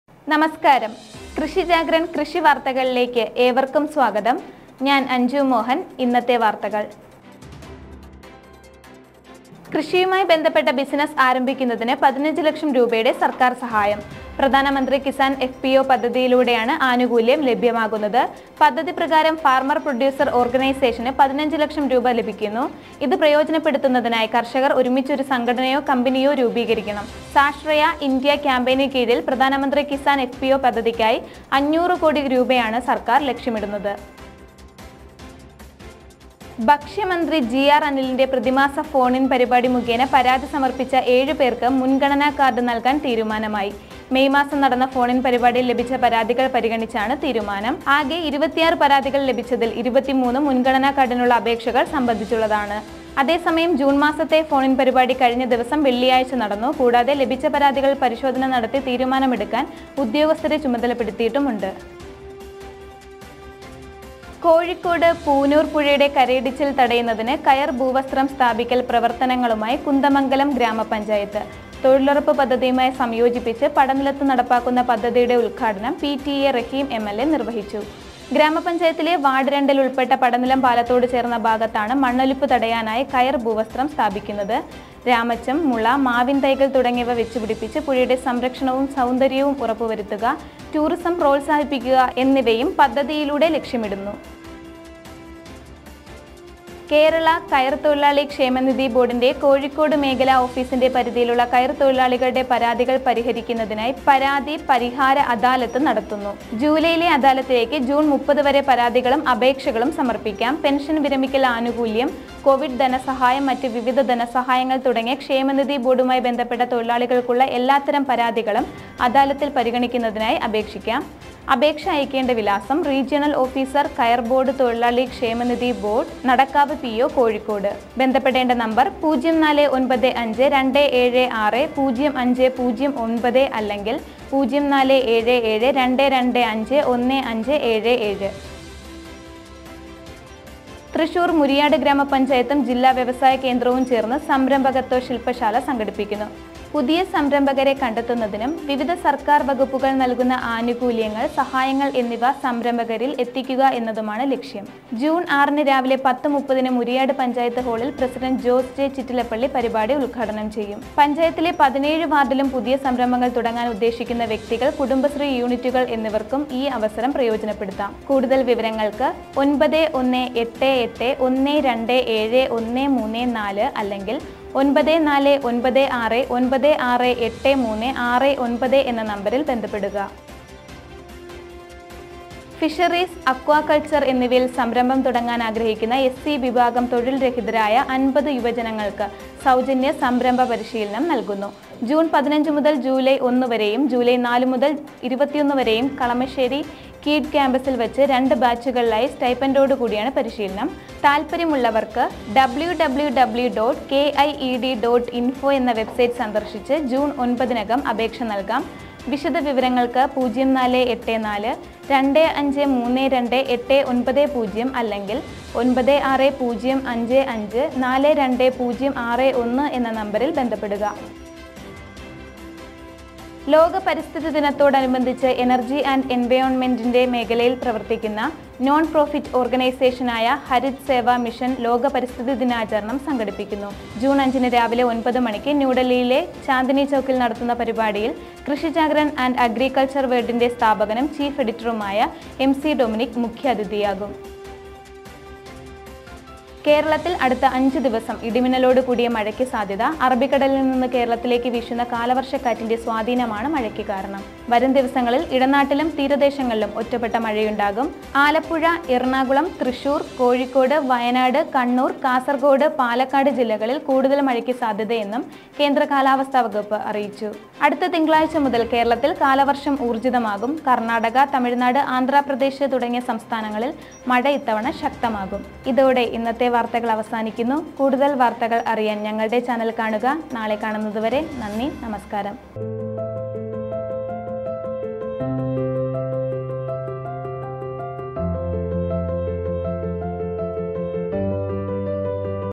PM Kisan FPO Scheme: 15 ലക്ഷം രൂപയുടെ സർക്കാർ സഹായം| PMKisan|Rationcard|environmentday|farmersgroup
കൃഷിയുമായി ബന്ധപ്പെട്ട ബിസിനസ്സ് ആരംഭിക്കുന്നതിന് 15 ലക്ഷം രൂപയുടെ സർക്കാർ സഹായം. Pradhan Mantri Kisan FPO Schemeലൂടെയാണ് ആനുകൂല്യം ലഭിക്കുന്നത്. പദ്ധതി പ്രകാരം Farmer Producer Organization 15 ലക്ഷം രൂപ ലഭിക്കുന്നു. ഇത് പ്രയോജനപ്പെടുത്തുന്നതിനായി, കർഷകർ ഒരുമിച്ച് ഒരു സംഘടനയോ കമ്പനിയോ രൂപീകരിക്കണം. സ്വാശ്രയ ഇന്ത്യ കാമ്പെയ്‌നിന് കീഴിൽ Pradhan Mantri Kisan FPO Schemeനായി 500 കോടി രൂപയാണ് സർക്കാർ ലക്ഷ്യമിടുന്നത്.